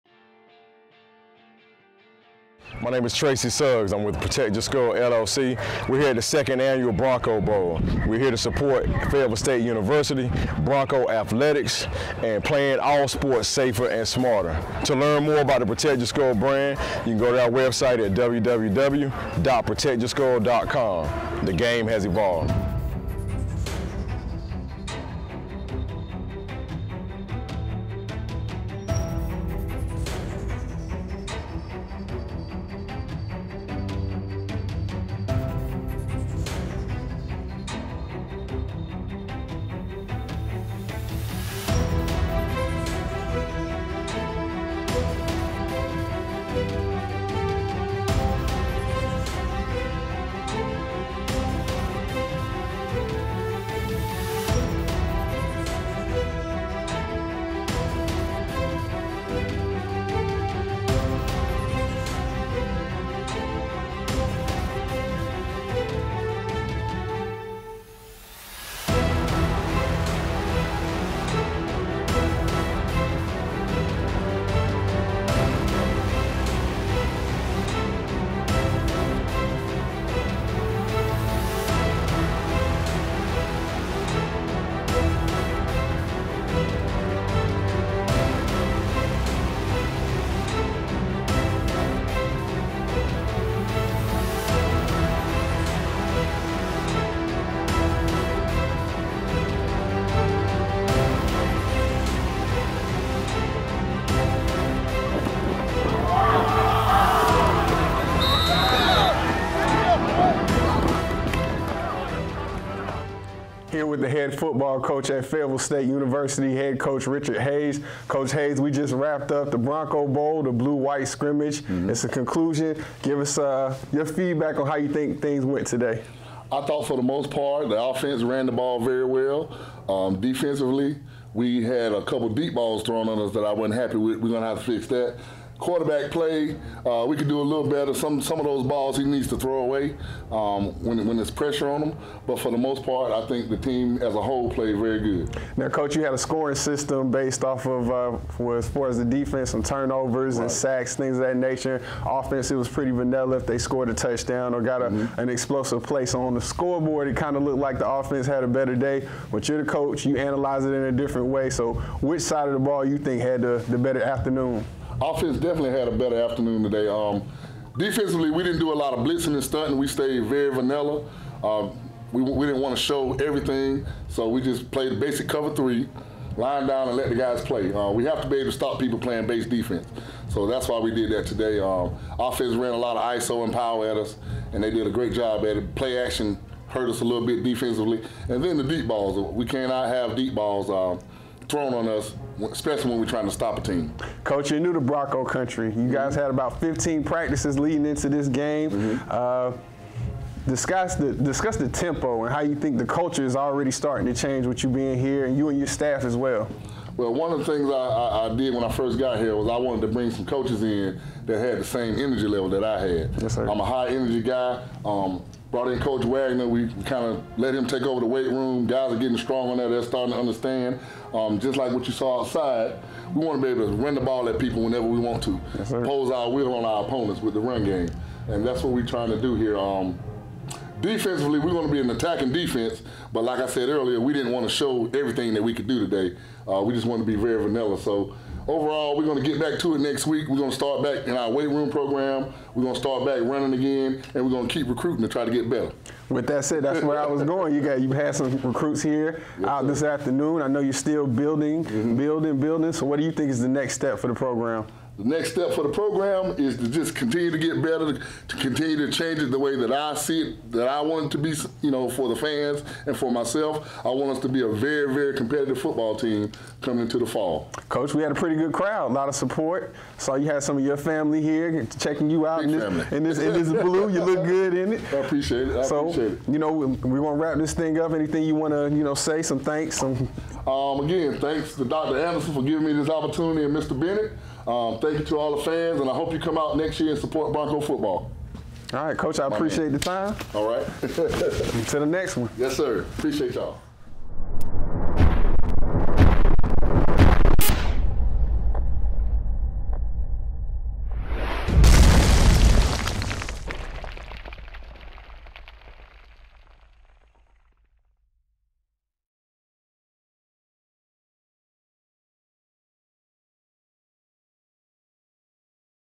2nd Annual Bronco Bowl Recap (FSU Football)
Fayetteville State University Head Football Coach, Richard Hayes​ and SID, Lamont Hinson, break down some of the highlights from the 2nd annual FSU Bronco Bowl held on April 9th at Luther "Nick" Jeralds Stadium​ on the campus of Fayetteville State University​!

Executive Produced by: Protect Your Skull, LLC.​

FSUBroncoBowl‪ #‎BroncoPride‬ ‪#‎BuiltBroncoTough‬ ‪#‎1DayBetter‬ ‪#‎ResNonVerba‬ ‪#‎HBCU‬ ‪#‎CIAA‬ ‪#‎NCAAD2‬ ‪#‎MakeFSUYours‬ ‪#‎AttitudeCheck‬ ‪#‎BleedBlue ProtectYourSkull ‪#‎SportsVideography‬ ‪#‎SportsPromo‬ ‪#‎TeamEiPic‬ ‪#‎TheEiPic‬